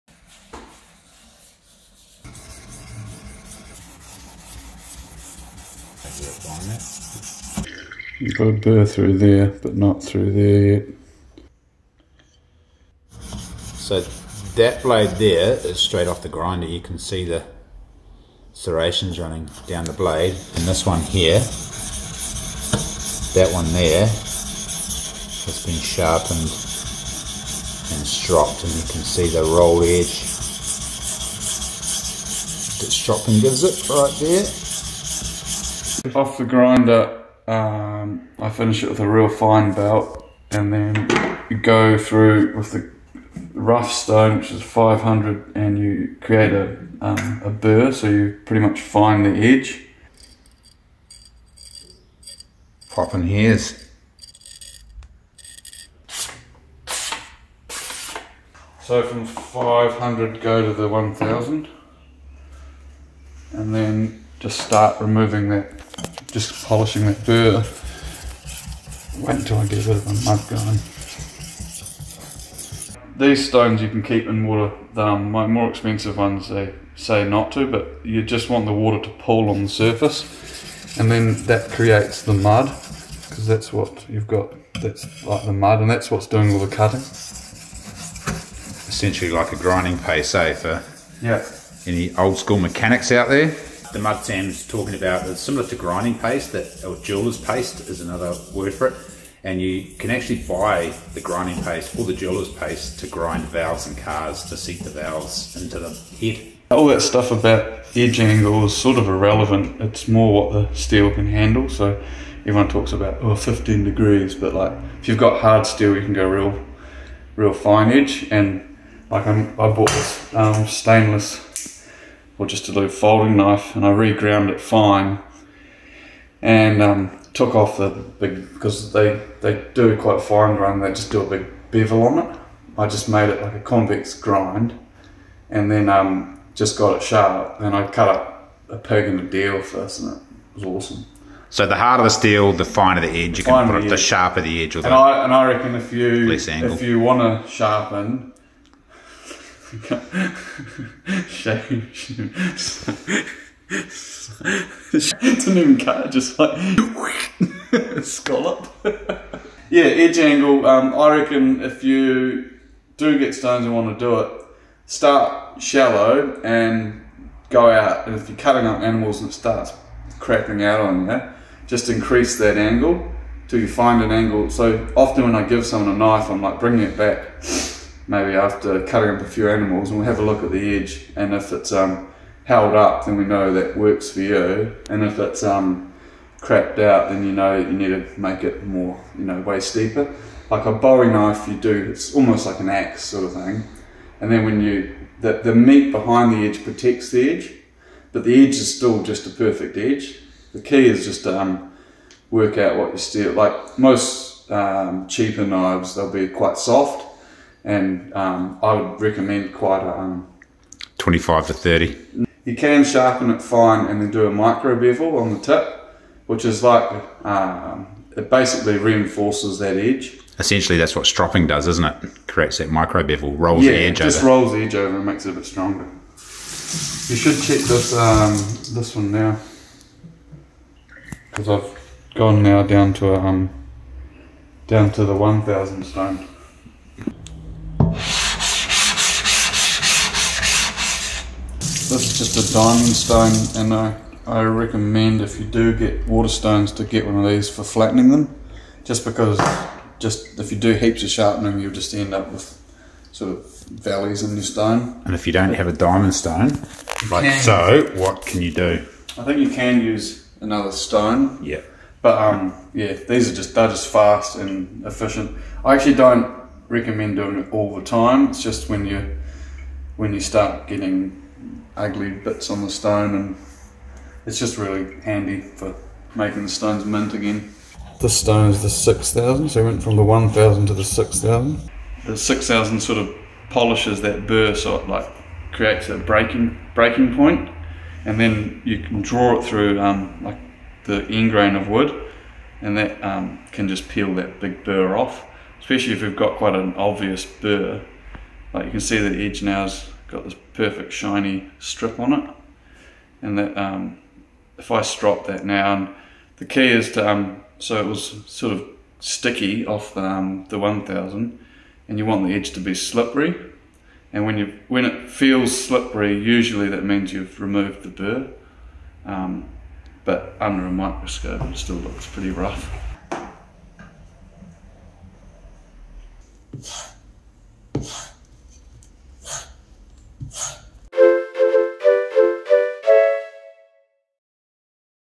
how to sharpen a knife RAZOR SHARP using a whetstone/waterstone with Sam Harrison Knives New Zealand
Sam demonstrates how to sharpen a knife for the best edge and the best results, stay sharper longer and cut cut cut your way to a happier life. knife sharpening 101